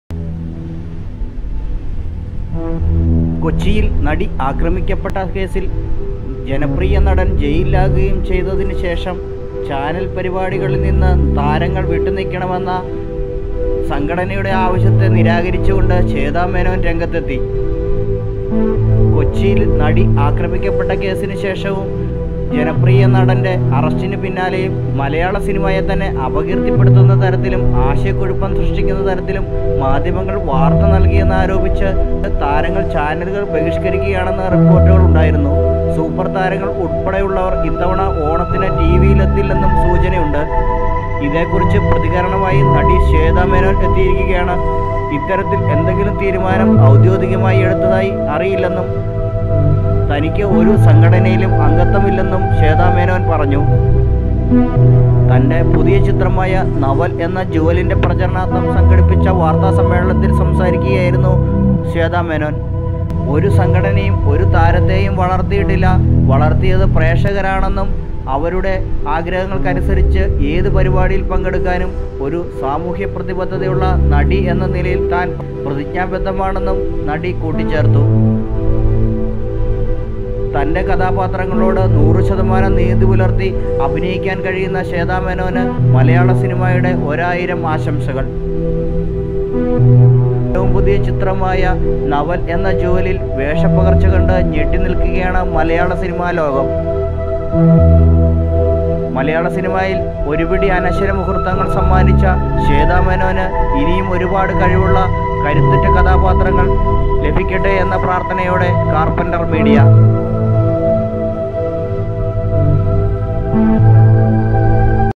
ശ്വേതാ മേനോൻ ഒരു ആൺകുട്ടീ തന്നെ.ഇങ്ങനെ പറയുന്നവർ സിനിമയിൽ ഉണ്ടോ?
ശ്വേതാ മേനോൻ കനത്ത മറുപടിയുമായി എല്ലാവരും അമ്പരന്നു. swetha menon#Carpenter Media Malayalam Film News &Gossips Malayalam Full Movie Malayalam Film2017